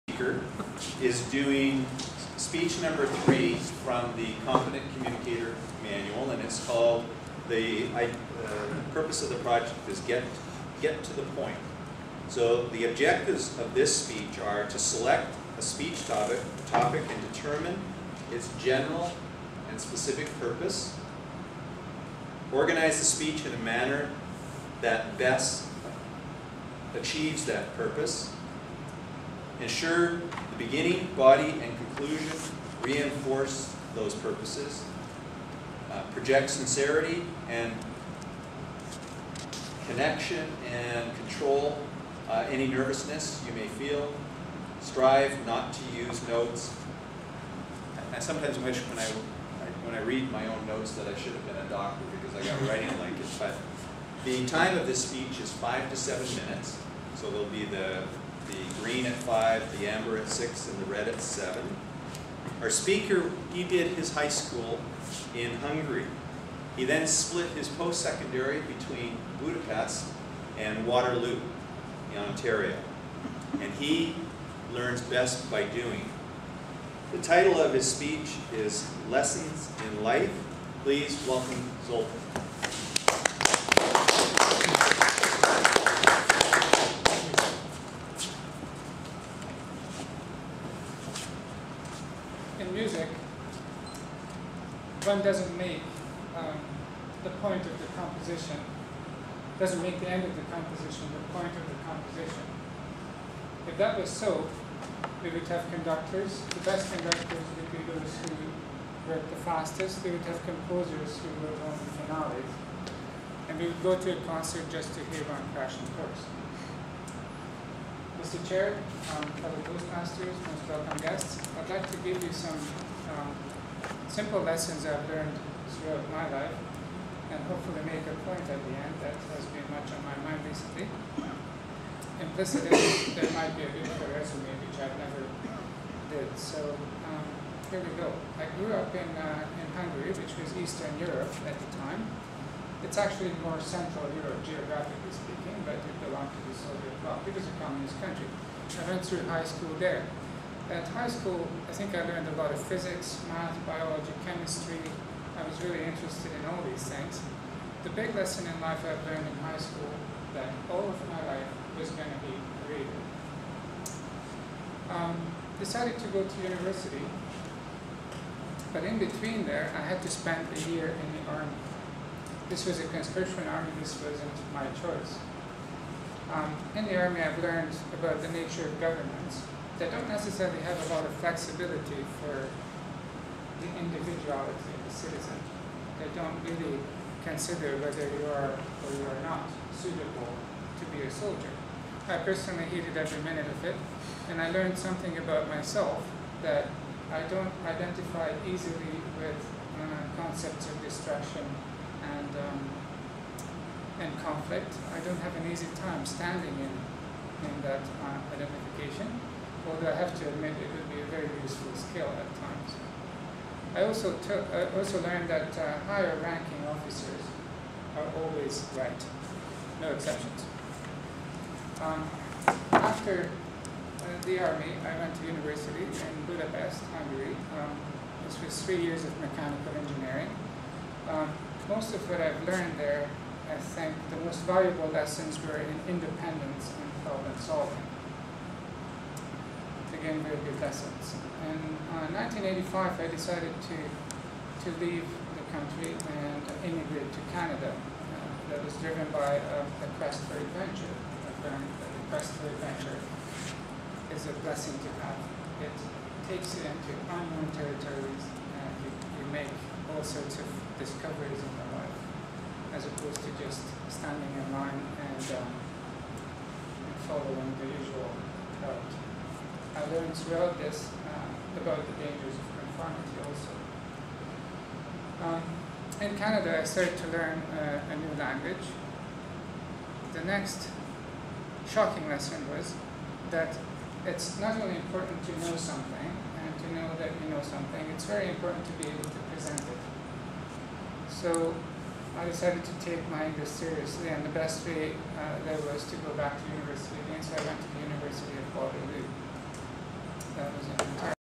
2010.5.27 - Zoltan's Speech Part 1
Competent Communication Manual
Speech #3: Get to the Point
Title: "Lessons in Life"